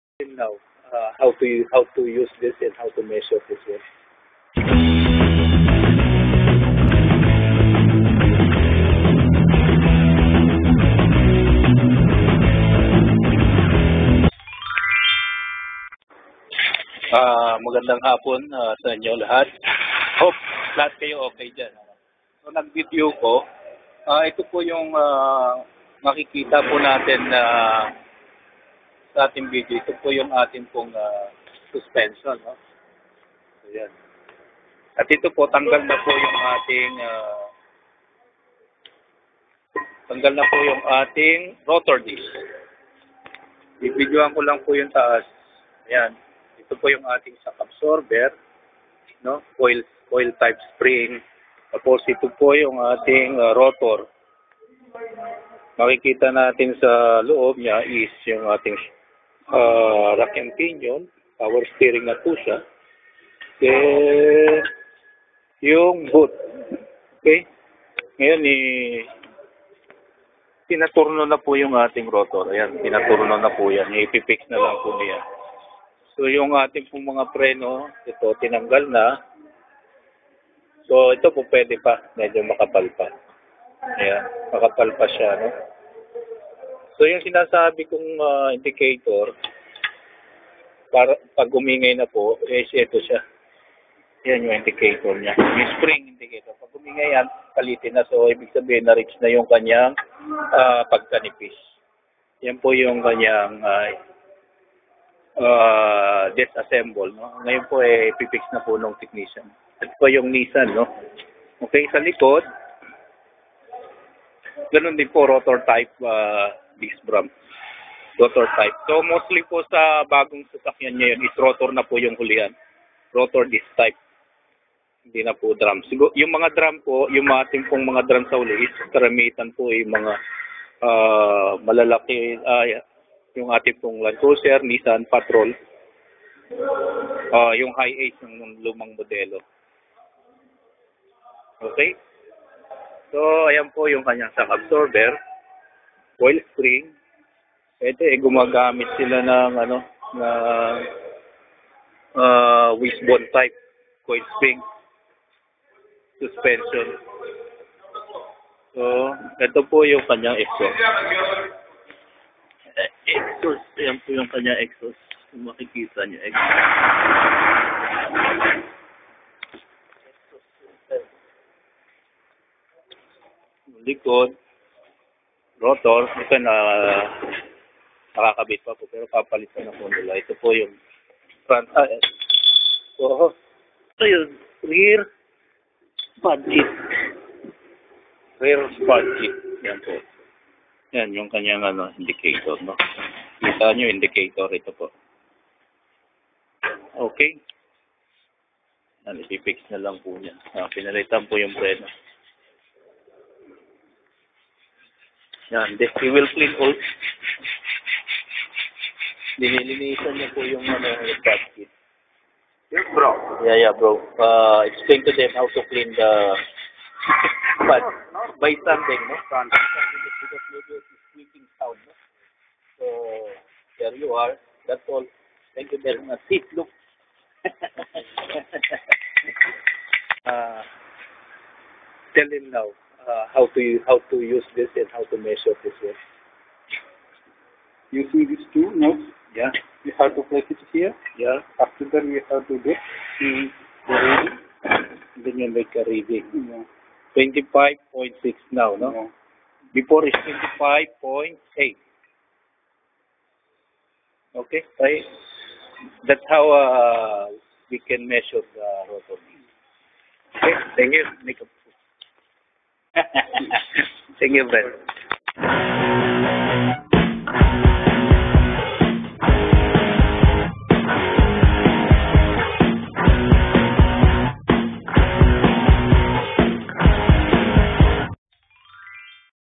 ROTOR DISC MACHINING (Brake maintenance)
This is to show you what is the Periodic maintenance did in brake system
and Measuring Rotor if necessarily need Machining or replaced